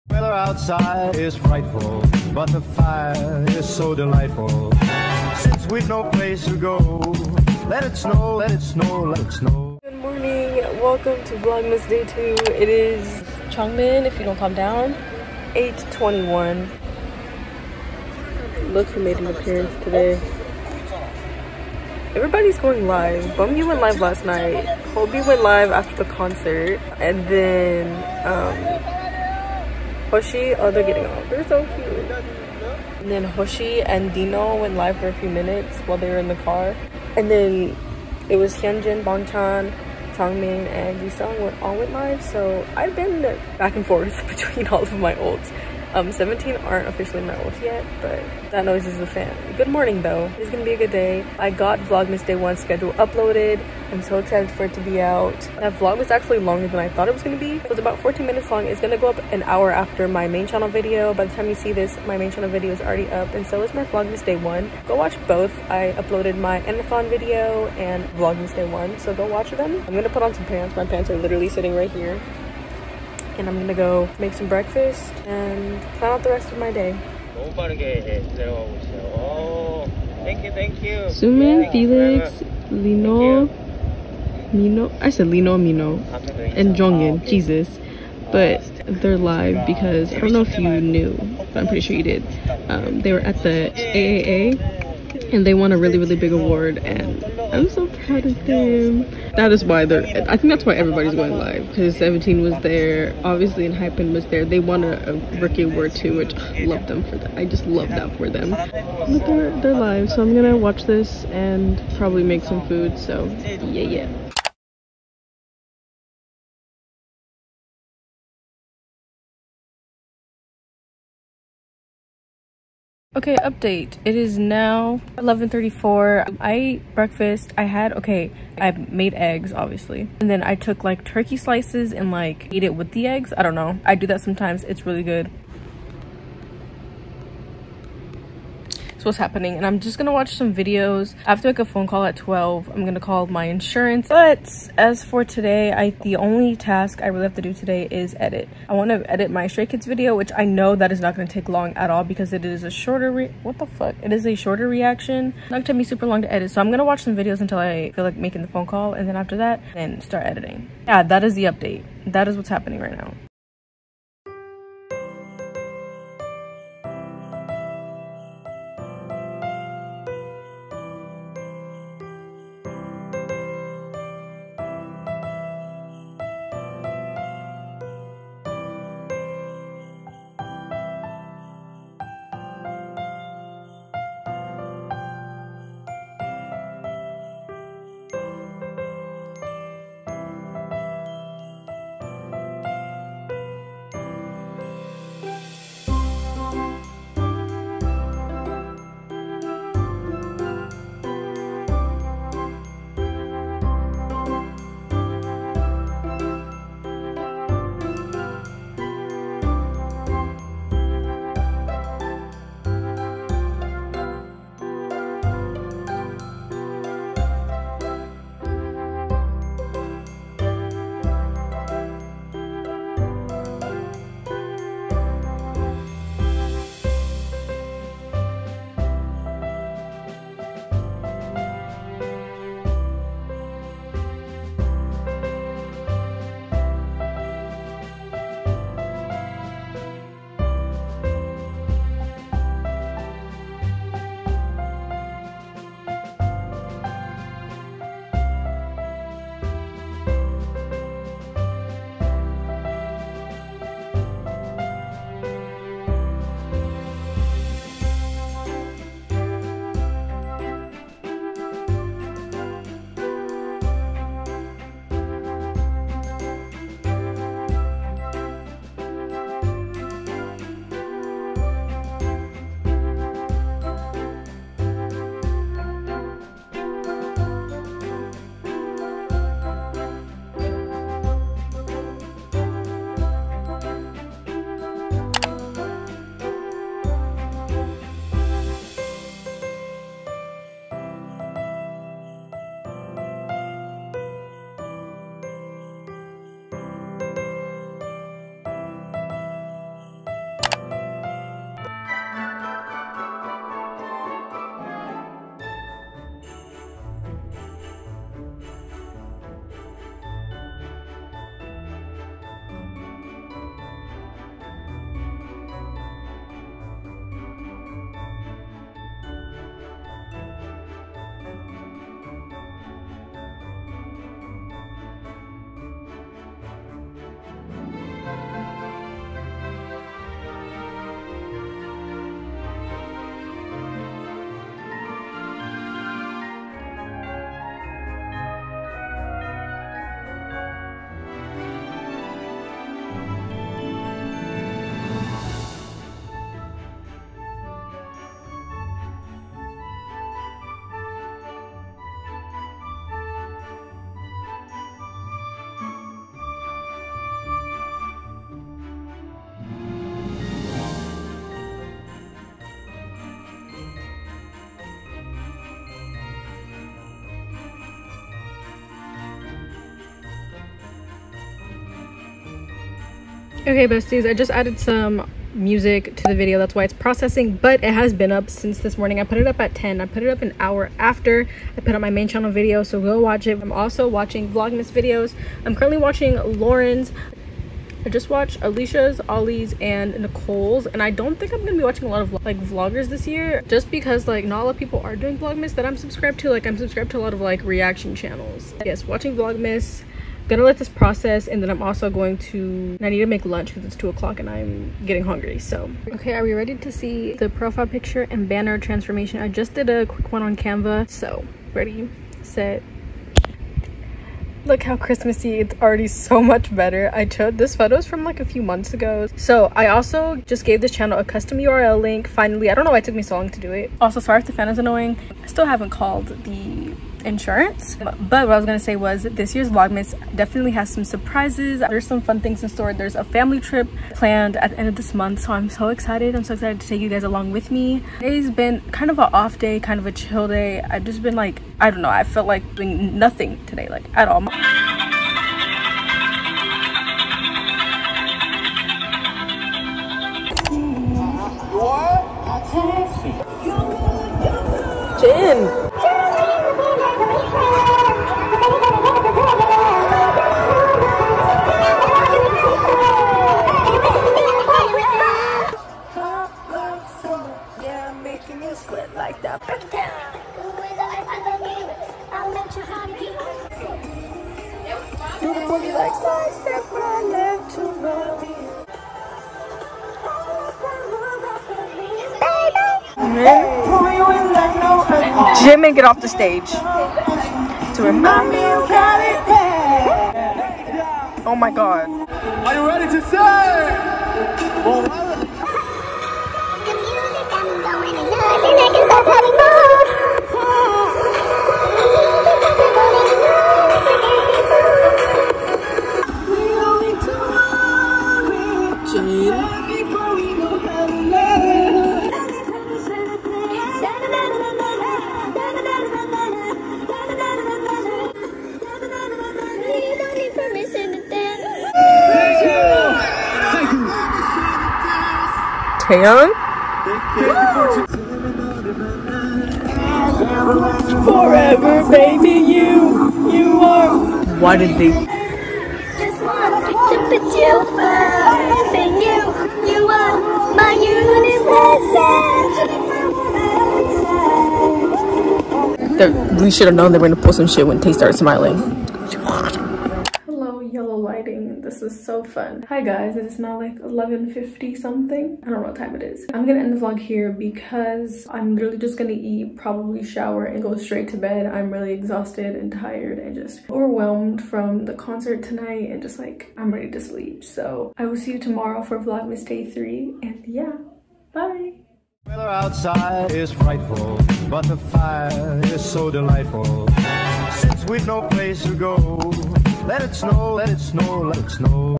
vlogmas day 2 ☆ wrapping christmas presents, vlive, & ptd on stage day 4!
vlogmas day 2 ❄️🎄🎁☃️ merry vlogmas lovies!!! here is day 2 where i wrapped a christmas present, watched a few vlives and enjoyed bts’ last show in la :(( . i hope u enjoyed !! ♥️